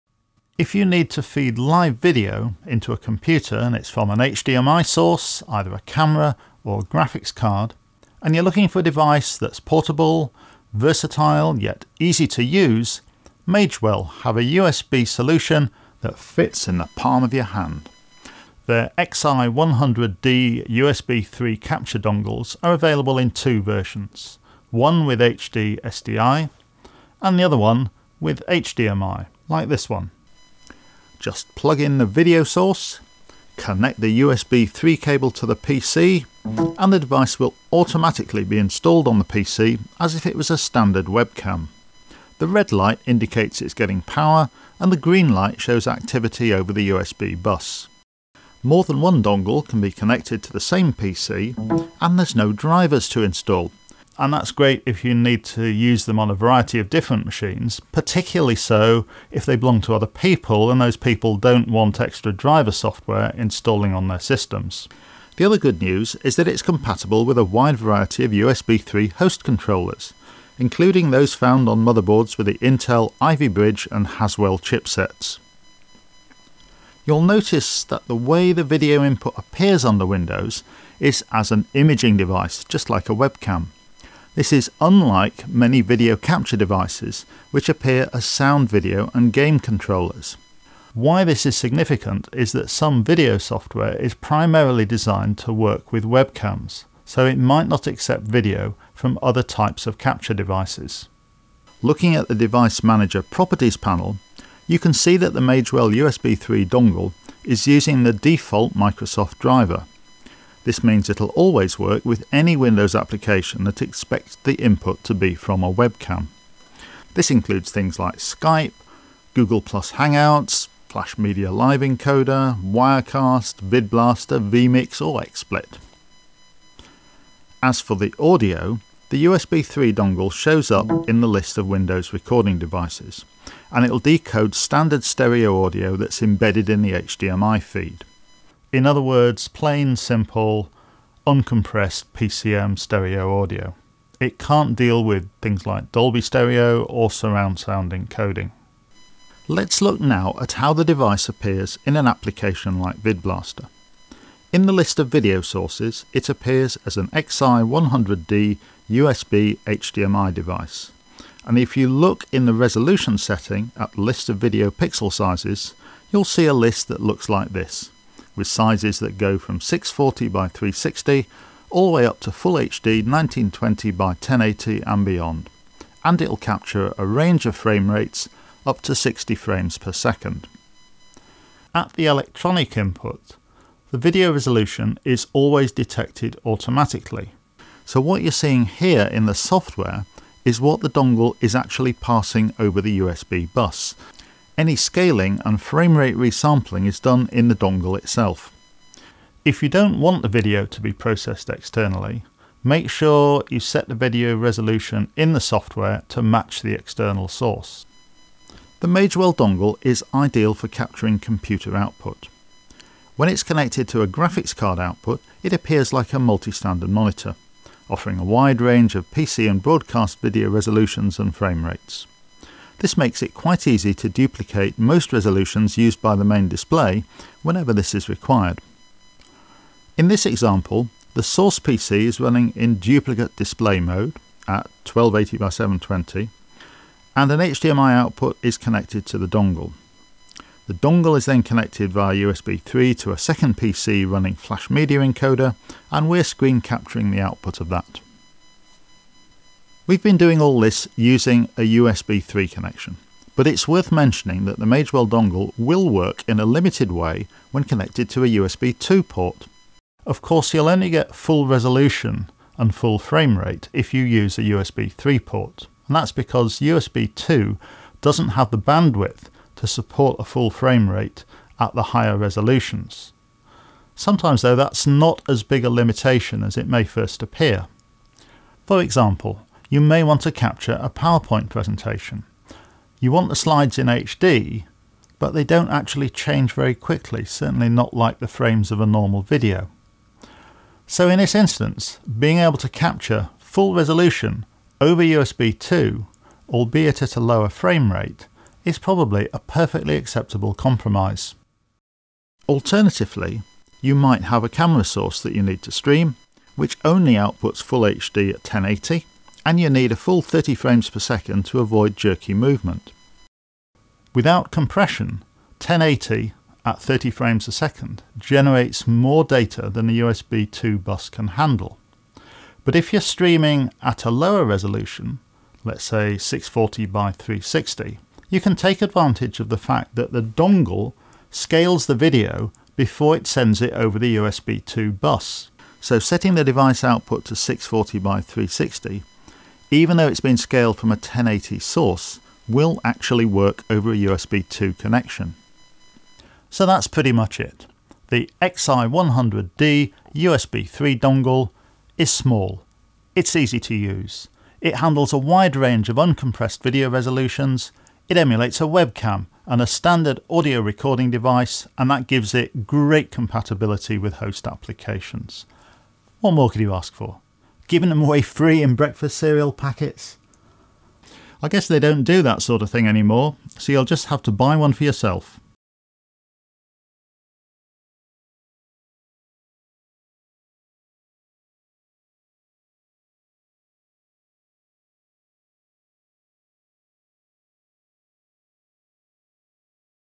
Magewell XI100D USB3 Dongle HDMI Video Capture Device
An introduction to the Magewell XI100D USB3 Video Capture Dongle, looking specifically at the HDMI version under Windows. (There's also a HD-SDI version, and it works on Macs too). The video shows it working in apps like Skype, Wirecast, FMLE, VidBlaster, XSplit, and explains why it's got such good compatibility with any application that is designed to work with a webcam. Embedded audio is also available as a standard Windows audio recording device, as well as the HDMI video, which can be from SD or HD cameras, or from a PC graphics card. Although designed for a USB3 connection, the device works - with some limitations - when connected via USB2.

In 2015 Magewell redesigned the casing of these dongles so that they are now silver metal rather than black plastic, and the USB3 connector is larger (and stronger). Aside from that, they are much the same. There is now a software application called Dongle Helper for testing the dongles and updating/repairing the firmware, as well as changing the hardware de-interlacing setting. You can see a video guide to using it

Note: While comments and questions about the dongle are welcome, if you need direct technical support then please use the form on the Magewell website. In particular, I can't troubleshoot why you may not be getting video or audio coming through from external hardware that I neither own nor have access to.